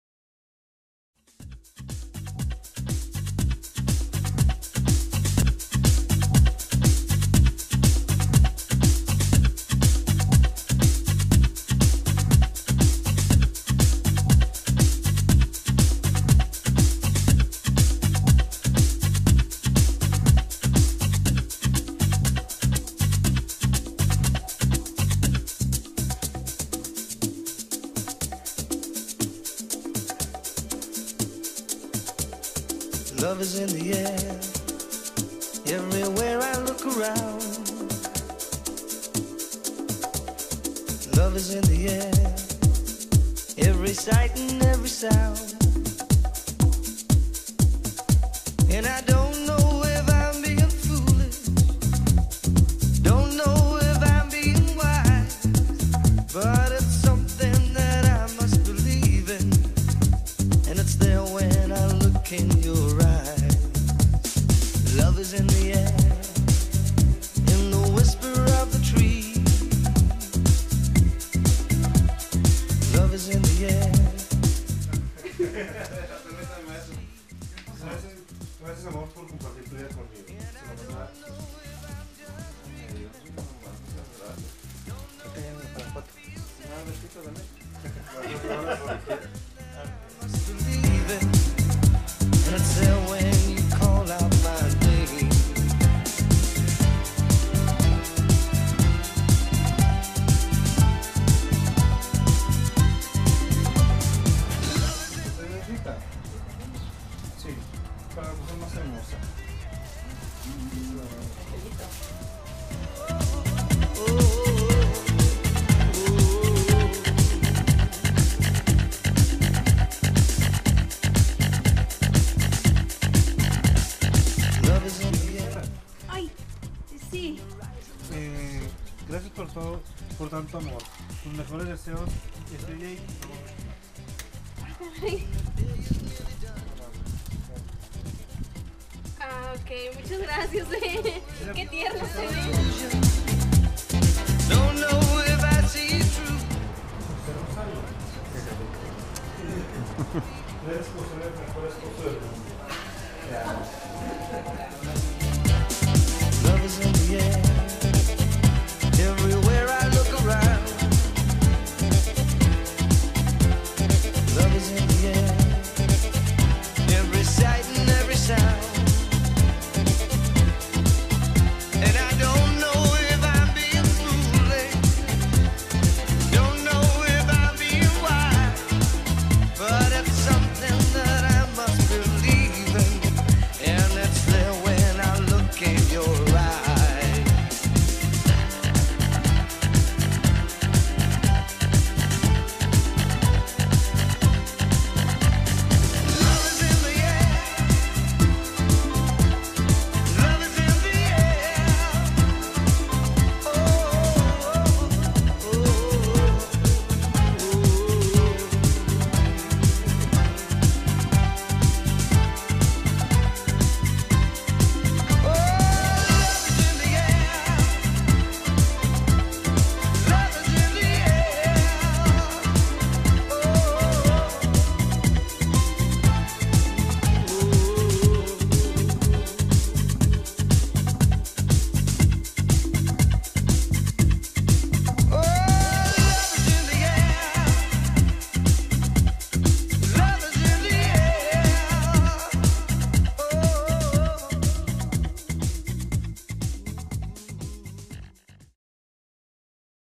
Cupido Volkswagen
Cupido Volkswagen salió a repartir felicidad a los Hermosillenses.